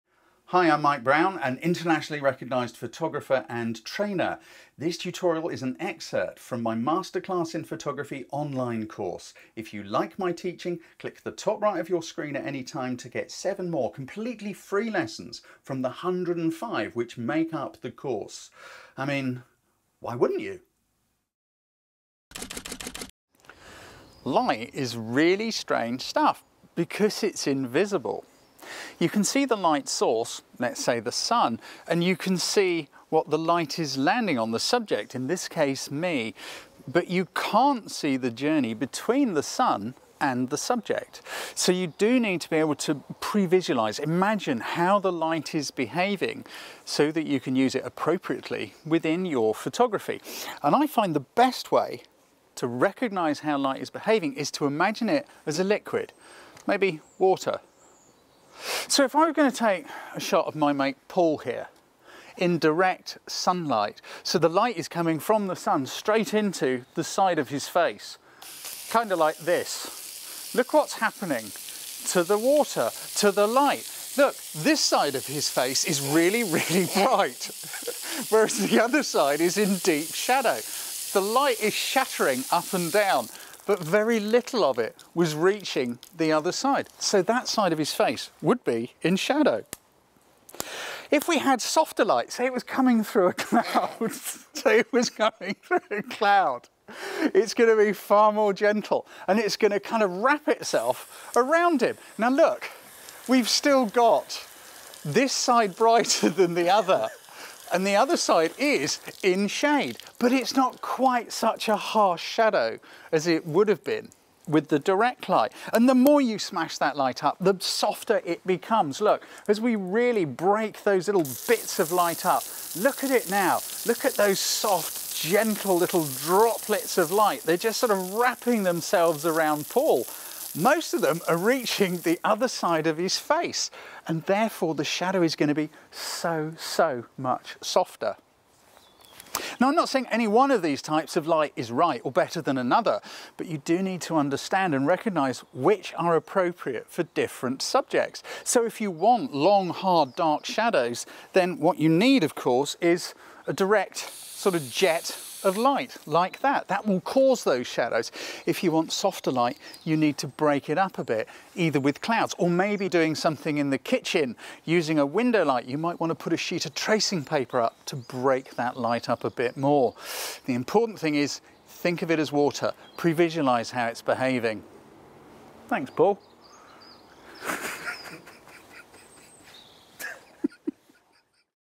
Qualities Of Light Explained - Mike Browne
In photography light qualities are the way light falls on the subject. Hard light makes bright highlights and dark shadows, flat light has neither. Diffuse light has some. As a photographer you must learn to see them and apply them appropriately to the subject of your image. All light is good light provided you use it well.